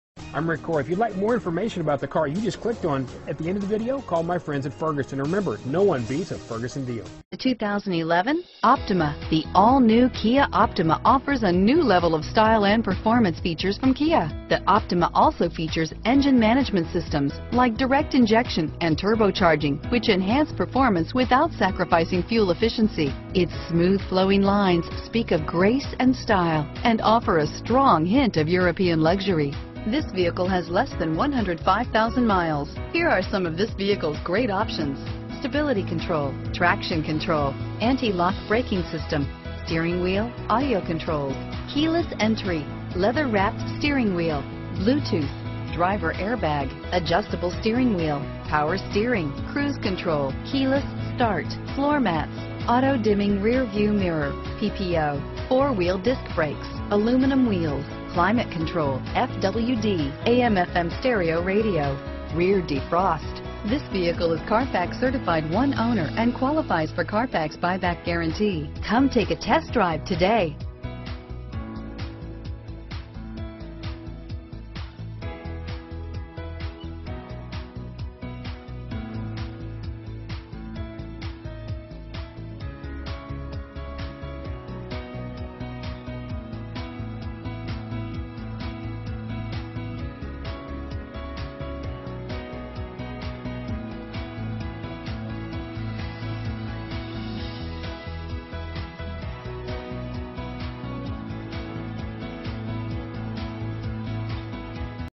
2011 Kia Optima Tulsa, Broken Arrow, Owasso, Bixby, Green Country, OK S6439A
2011 Kia Optima

Ferguson Superstore
1601 N Elm Place

Clean Carfax, One-Owner. ABS brakes, AM/FM radio: SIRIUS, Compass, Electronic Stability Control, Front dual zone A/C, Heated door mirrors, Illuminated entry, Leather Seat Trim, Low tire pressure warning, Remote keyless entry, and Traction control.How would you like riding off in this stunning 2011 Kia Optima at a price like this? Autoweek calls the Optima a car with elegant athleticism and a certain panache, a sedan that means business. Great condition is the one of the most desirable traits in an used vehicle, and this Optima is the definition of it.Ask for the Import Store when calling. Ferguson Advantage Imports. We've got what you're looking for.